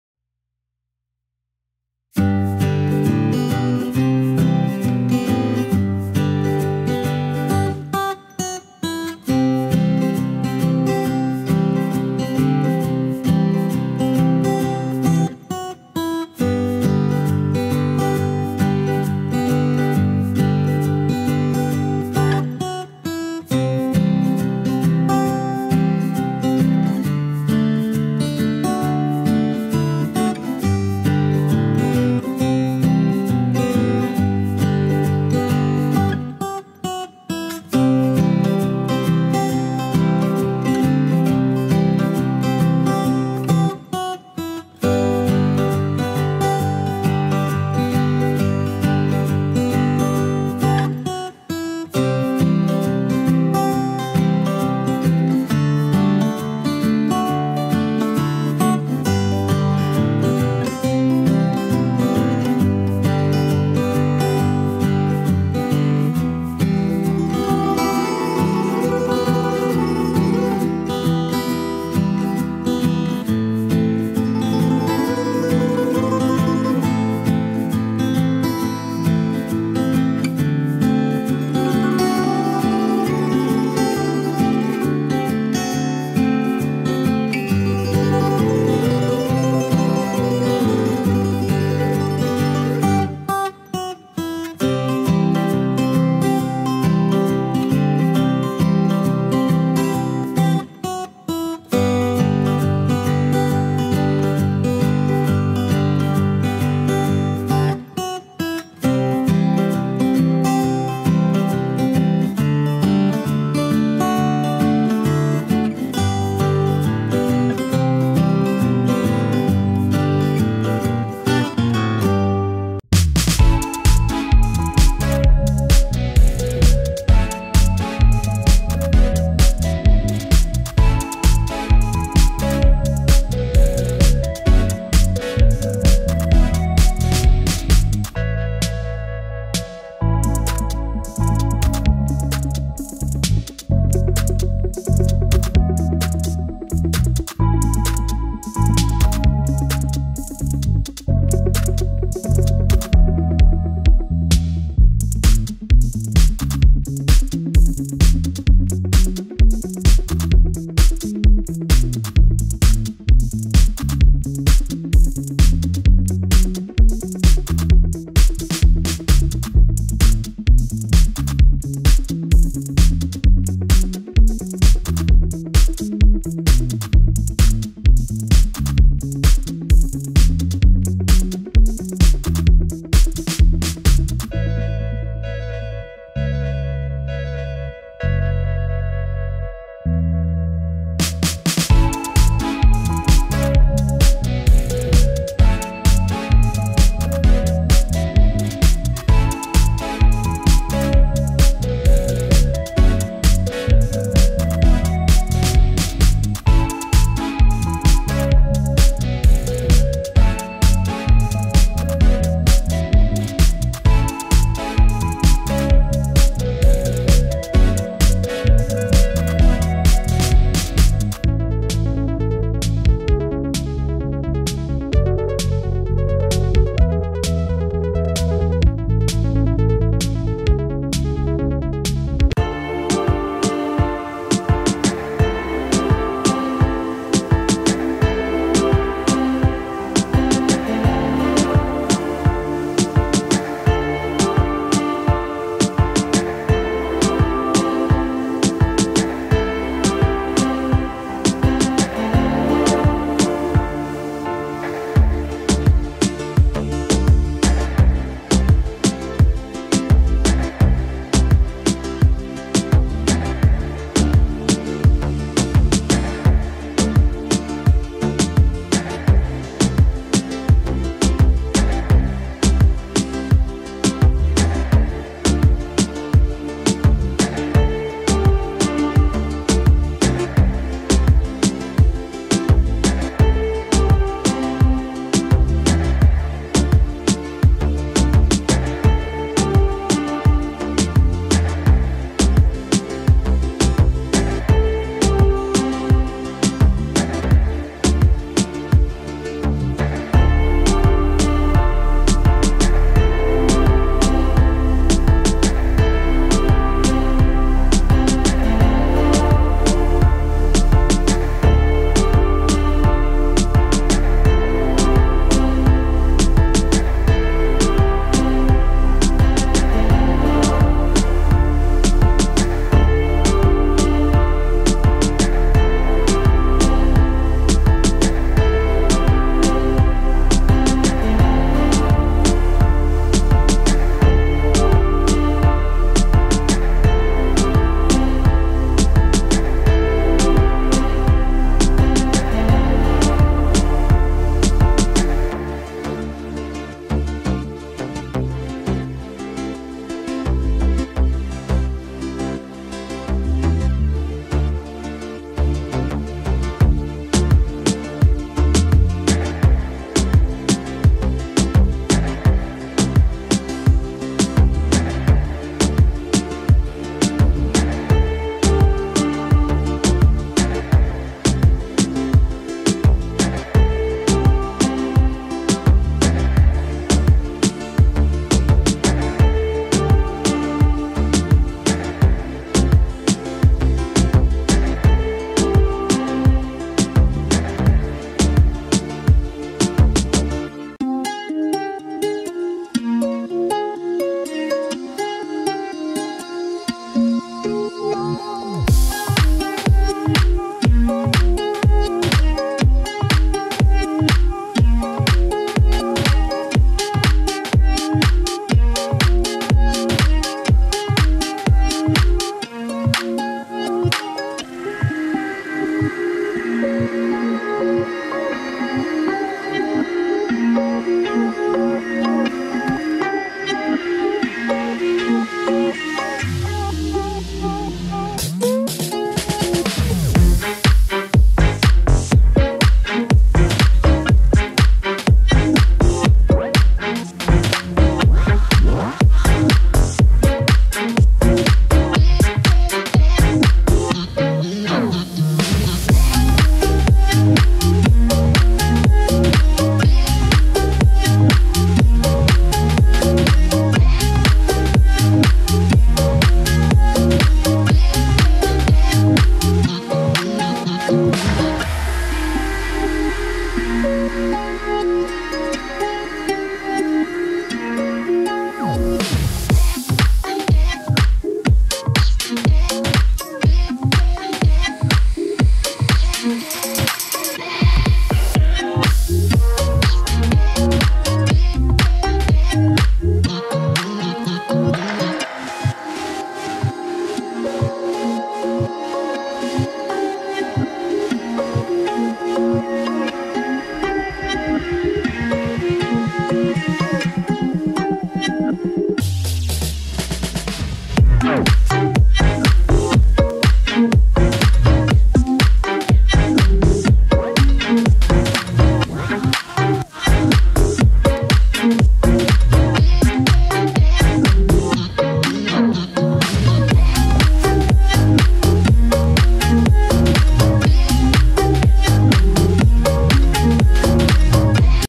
2 BEAUTIFUL HANDMADE FLOWER VASE OUT OF OLD COFFEE CUPS & MARBALL | DECORATION IDEAS
2 BEAUTIFUL HANDMADE FLOWER VASE OUT OF OLD COFFEE CUPS & MARBALL | DECORATION IDEAS